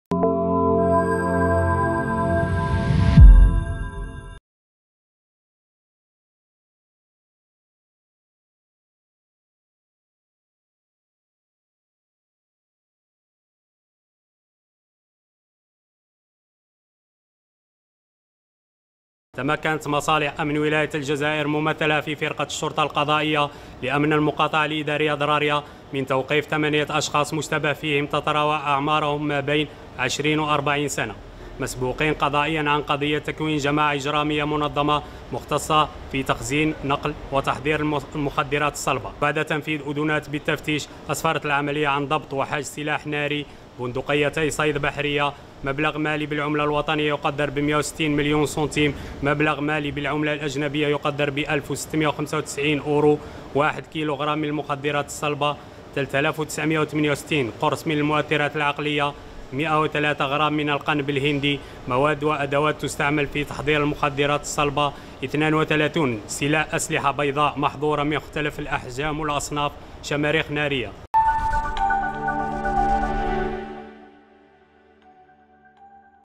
أمن ولاية الجزائر: تفكيك جماعة إجرامية مختصة في تخزين ونقل وتحضير المخدرات
الجزائر(وأج)- تمكنت فرقة الشرطة القضائية لأمن المقاطعة الإدارية الدرارية، من تفكيك جماعة إجرامية منظمة مختصة في تخزين ونقل وتحضير المخدرات الصلبة والمخدرات والمؤثرات العقلية لغرض البيع مع استرجاع أسلحة وكمية معتبرة من المخدرات، حسب ما أورده بيان لمصالح الأمن الوطني.